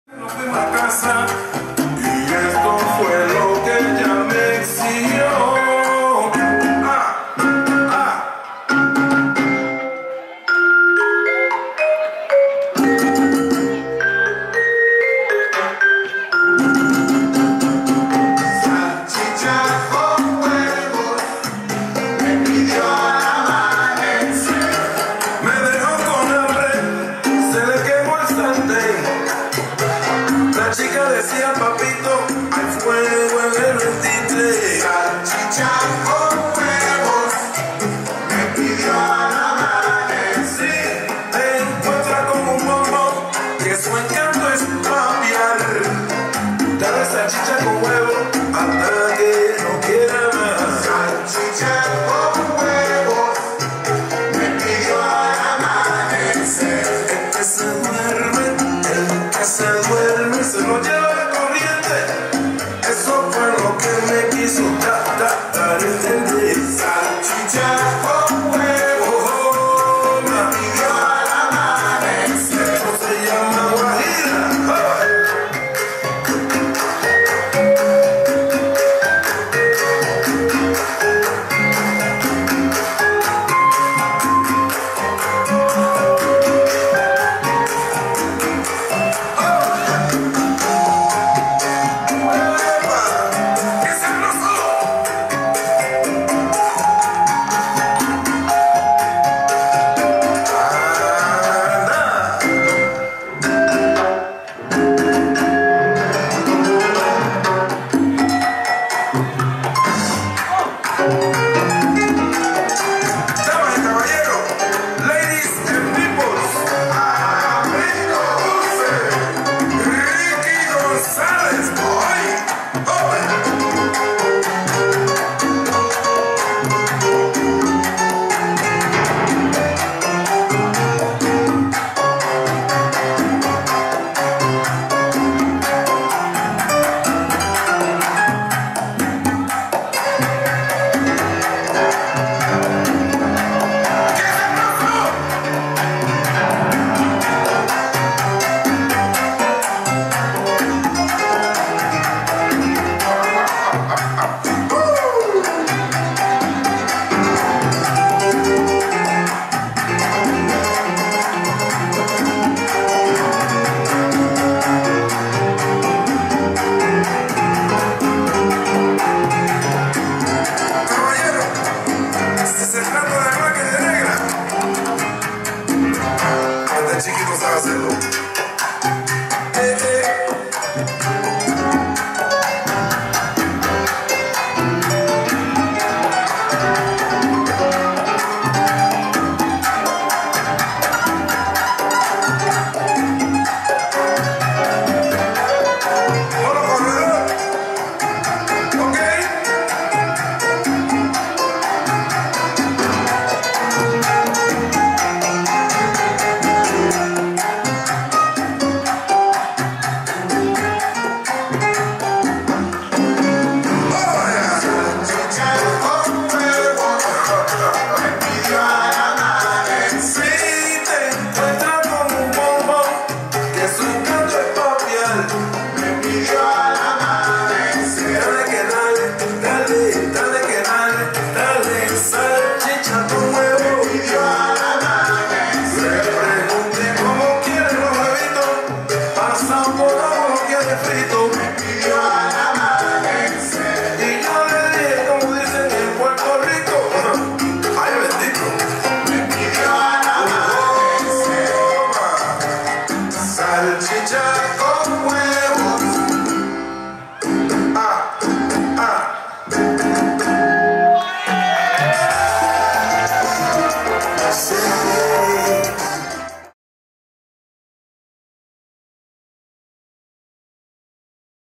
Terry (SalsAlianza) - Melisa Sahra Katılmış - Cha Cha Cha | LLF-2017
Terry (SalsAlianza) and Melisa Sahra Katılmış - Social Cha Cha Cha dance at Lebanon Latin Festival.